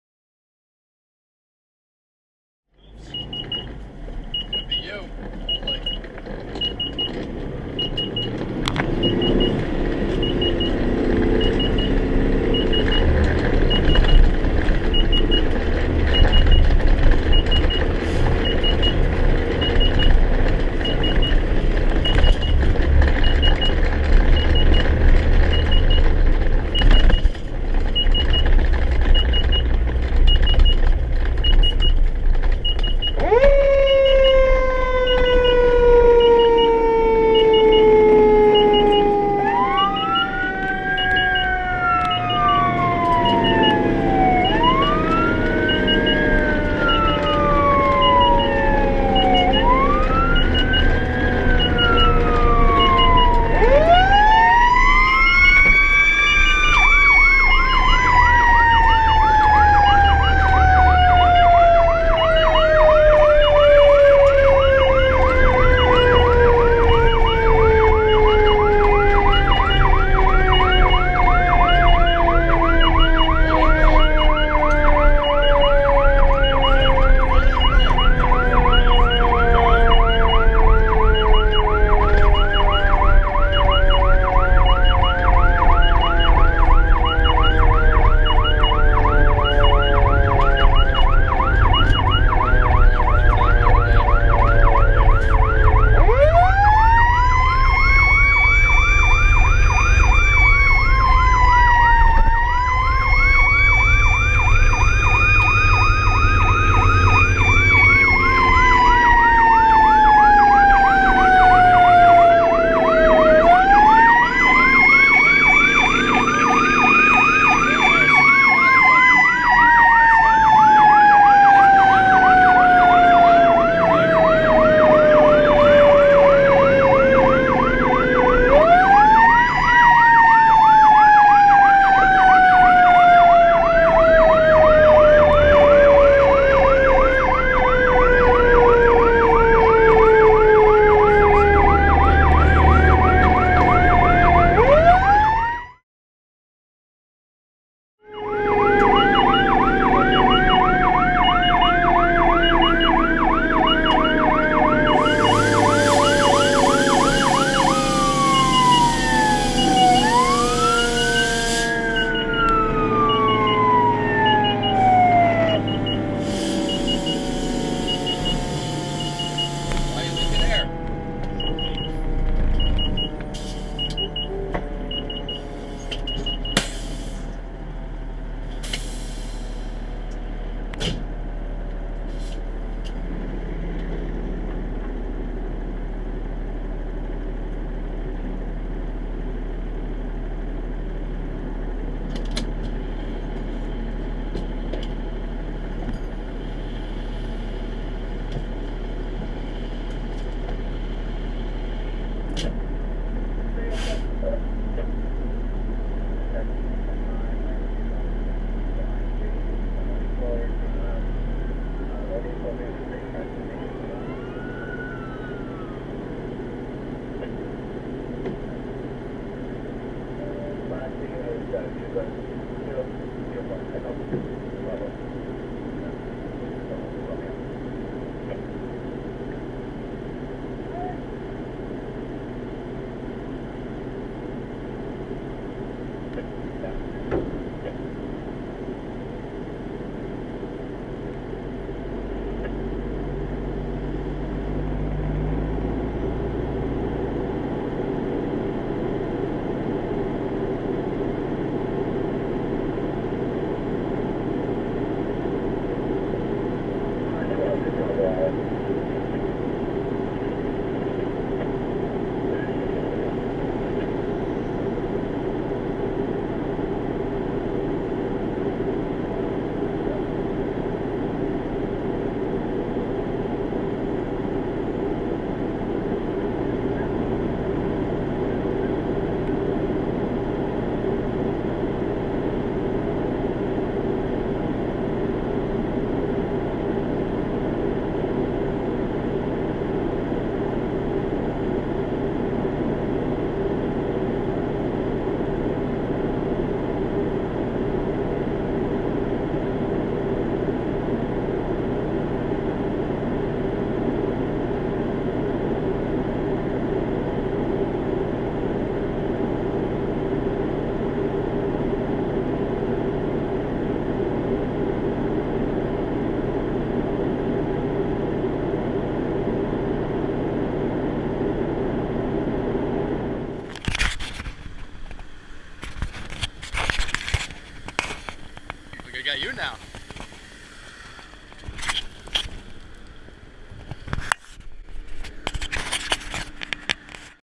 I-78 Car Fire
Mt Horeb Volunteer Fire Company responding to a car fire on I-78